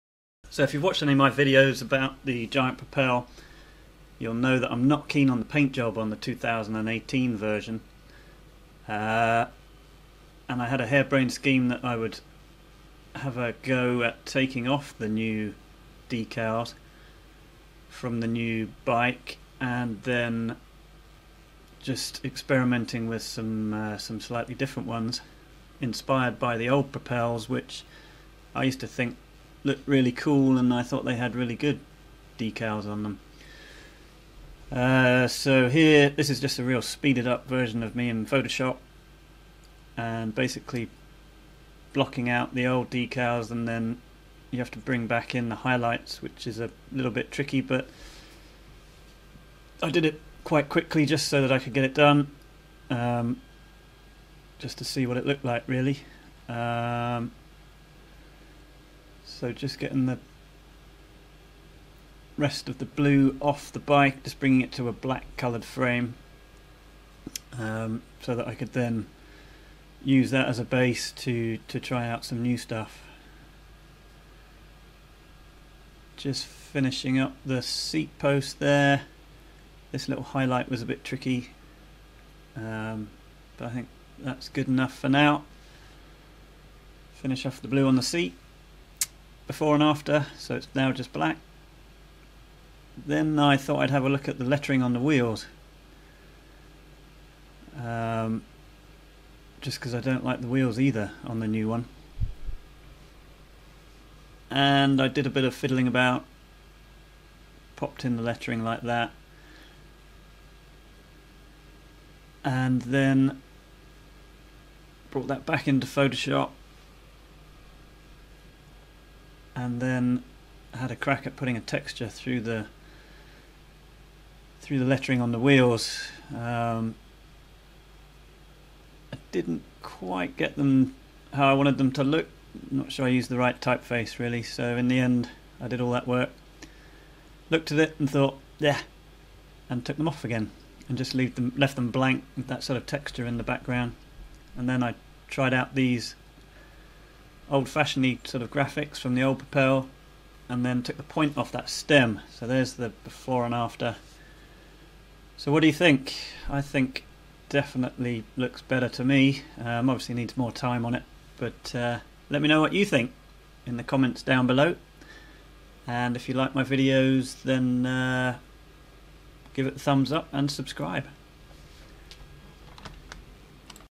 GIANT Propel 2018 Horrible paint job REMOVED!!
New Giant Propel 2018 - the top of the range model has an electric blue paint job which I don't like - being a fan of the old Propel retro paint job I wanted to see what the new bike would look like painted a bit like the old ones. So I took the new Propel 2018 into Photoshop, painted off the blue and then added some more retro graphics. I think it's an improvement, what do you think?